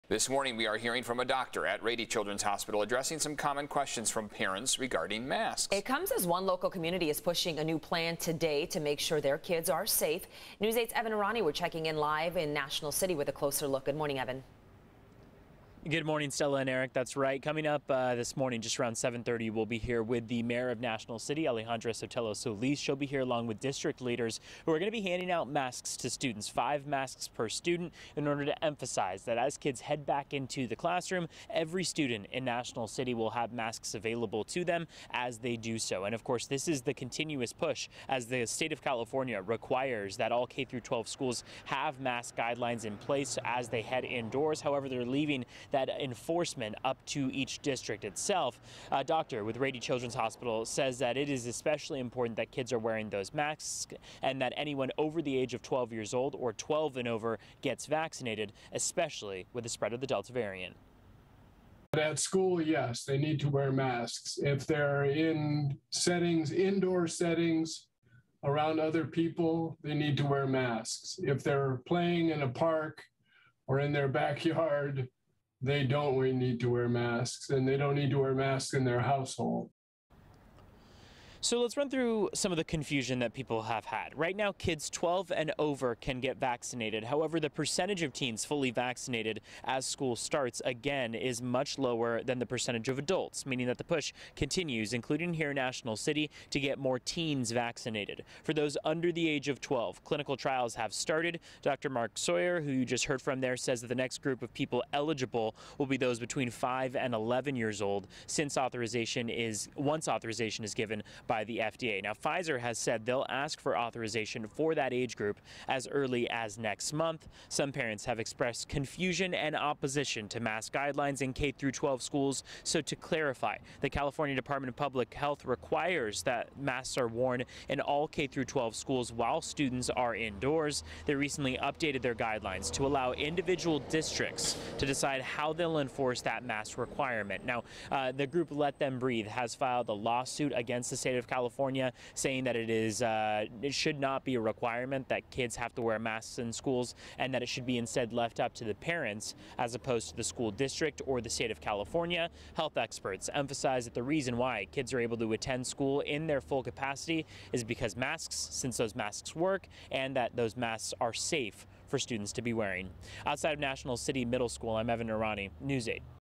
The latest on mask wearing in schools and vaccines for children under 12
"The parents should be the one's parenting and doing what's best for their family. The government can't seem to make a decision," said one parent.